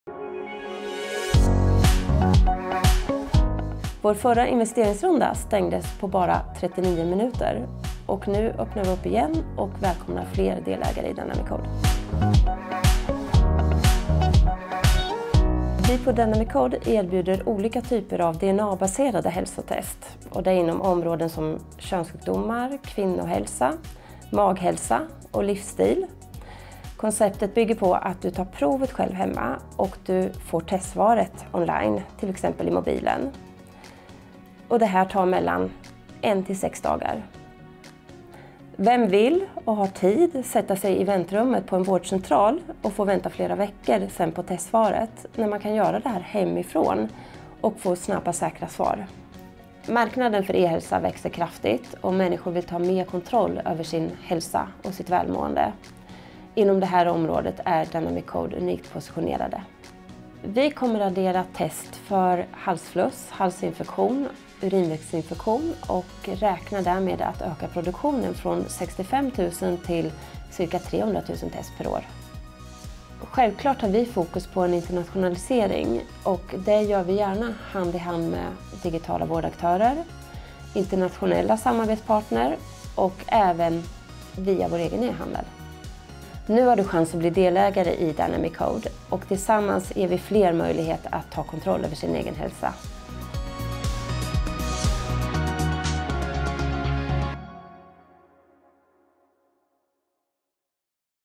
Dynamic Code
I en intervju med Finwire.tv berättar vd om bolaget, marknaden och planerna framåt.